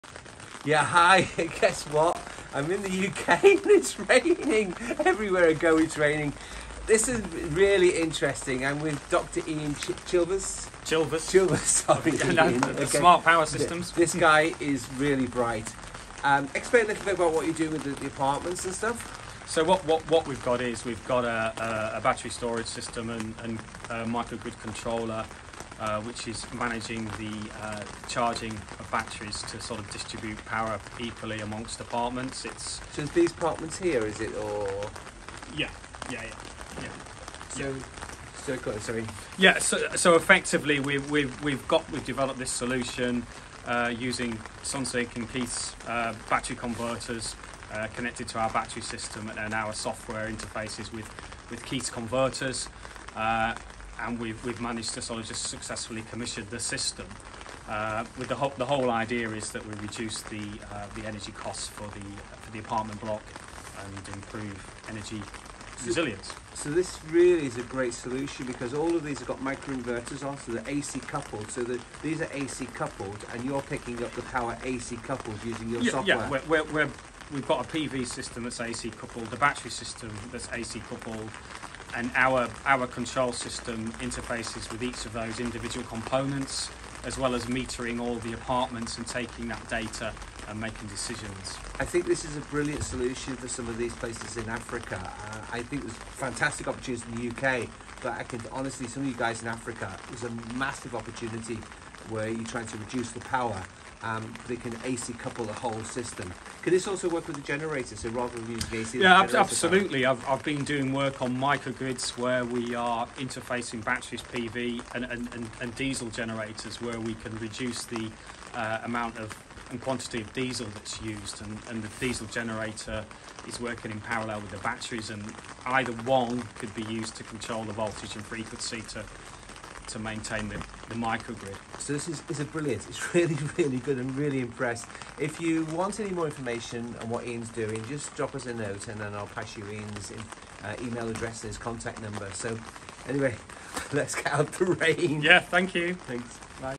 Micro-Grid Generation ( UK Style )
This is a project using Sunsynk inverters to save money on a  housing estate Using an ingenious piece of software developed by Dr Ian

Dr Ian Chilberts talks to Keith about his solar power system that revolves around a battery storage system managed by Sunsynk Inverters that equally distributes power amongst the apartments in the nearby development. The battery storage is AC coupled as is the PV system and when combined together provides an economic system that reduces the cost of mains power to each apartment.